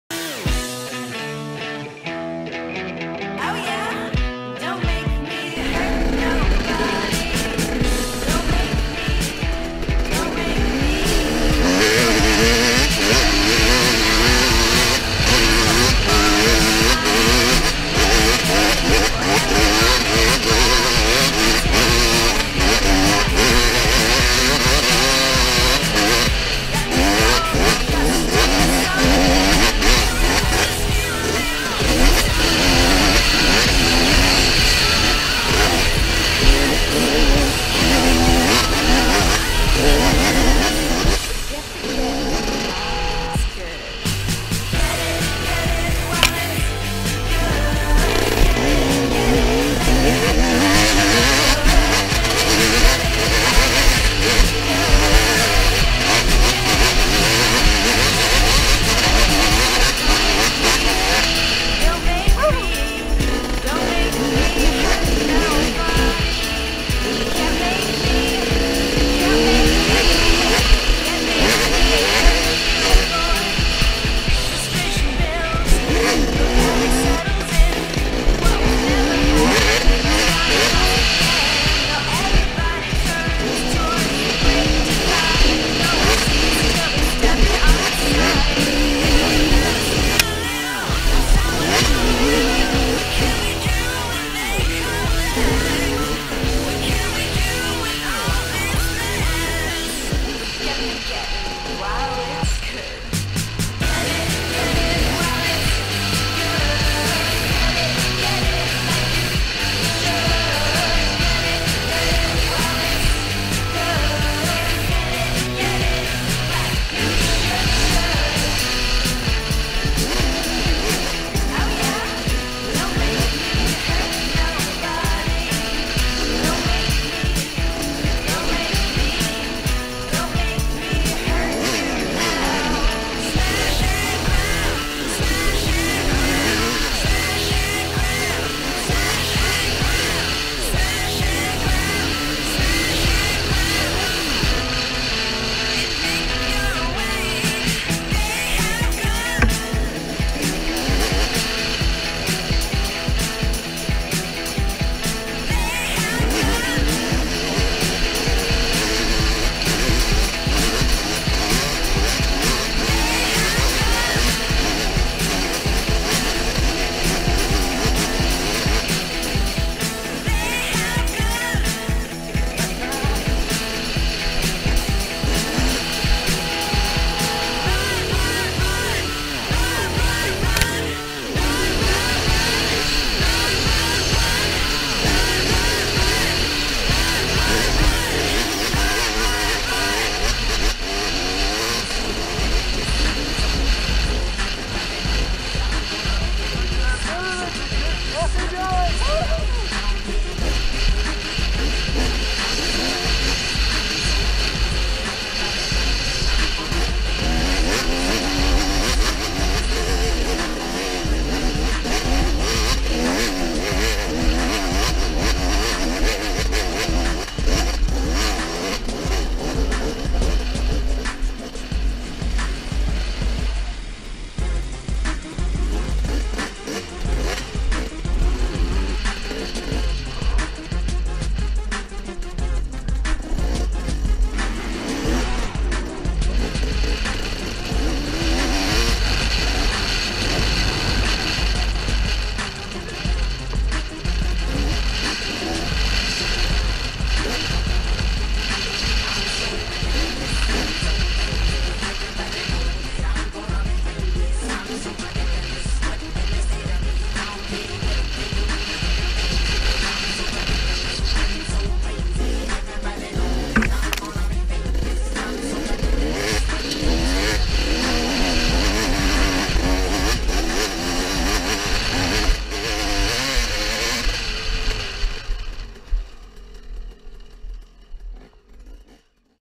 FOR THE LOVE OF HILLS! New Tracks - Maramarua Boxing Day Ride
video of a few of the hills at maramarua on boxing day